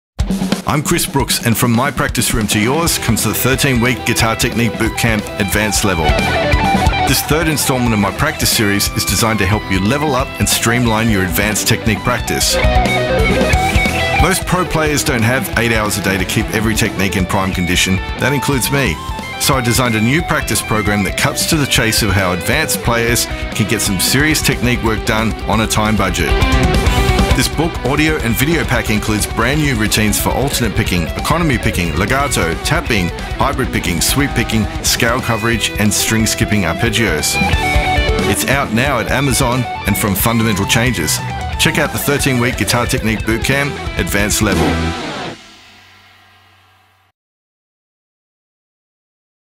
Out now: Thirteen Week Guitar Technique Bootcamp - Advanced Level
From my practice room to yours comes the Thirteen Week Guitar Technique Bootcamp – Advanced Level. This third installment of my practice series is designed to help you level up and streamline advanced technique practice.

Most pro players don’t have eight hours a day to keep every technique in prime condition. That includes me! So I designed a new practice program that cuts to the chase of how advanced players (and those aspiring to be) can get some serious technique work done on a time budget.

This book, audio and video pack introduces brand new routines for alternate picking, economy, picking, legato, tapping, hybrid picking, sweep picking, scale coverage, and string skipping arpeggios.

This targeted approach to practice homes in on the pressure points of each technique and delivers challenging musical exercises to address them.
With as little as thirty minutes per day, you’ll make gains in the practice room with a mix of “Tabata” and “tempo training” concepts, and I’ll even give you some templates to create a practice structure that works best for you.

With more than one hundred exercises and a final-boss study solo at the end, this advanced bootcamp is the perfect way to condense all the techniques taught in my other books into a manageable plan for progress and maintenance.

With expert advice, audio of every lick, and bonus video selections of many examples, this book represents sensational value from just $10. So place a bet on yourself, that you can become a better player in thirteen weeks.

The Thirteen Week Guitar Technique Bootcamp – Advanced Level is out now in paperback and Kindle from Amazon, or PDF from Fundamental Changes.